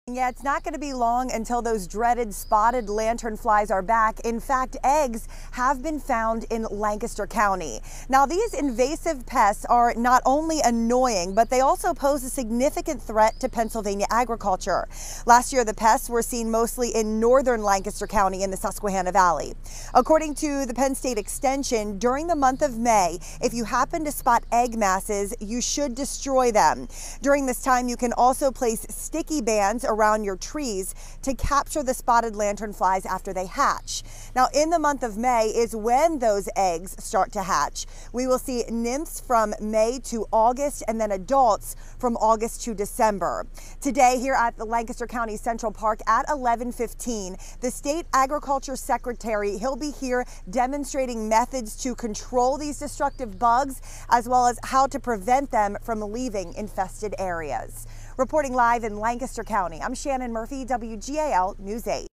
Pennsylvania's fight to stop the invasive Spotted Lanternfly
It won't be long until those dreaded Spotted Lanternflies are back.